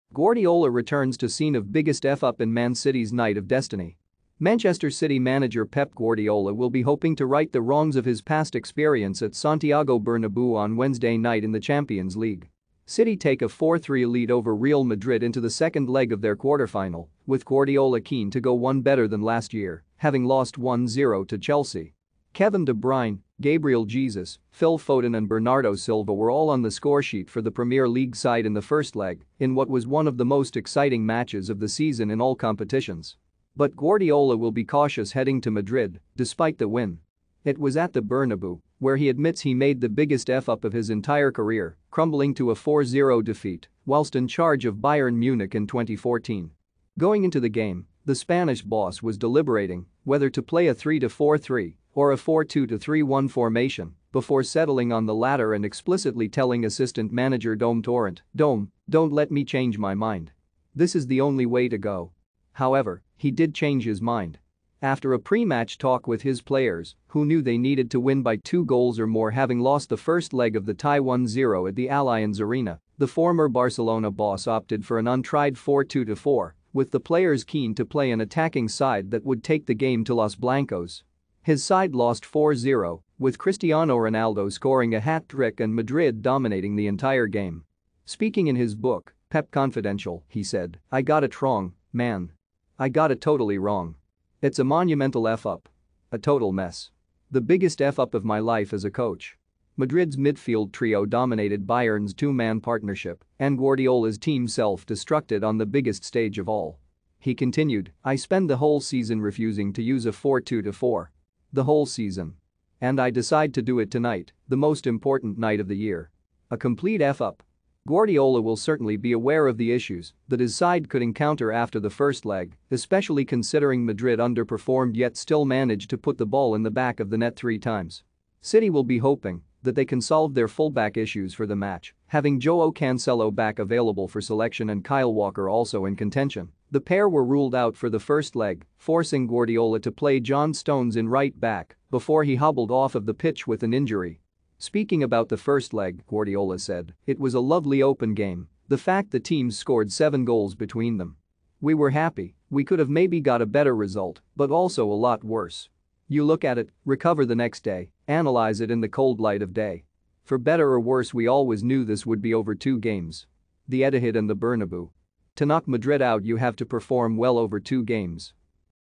Guardiola returns to scene of "biggest f*** up" in Man City's night of
Guardiola returns to scene of "biggest f*** up" in Man City's night of destiny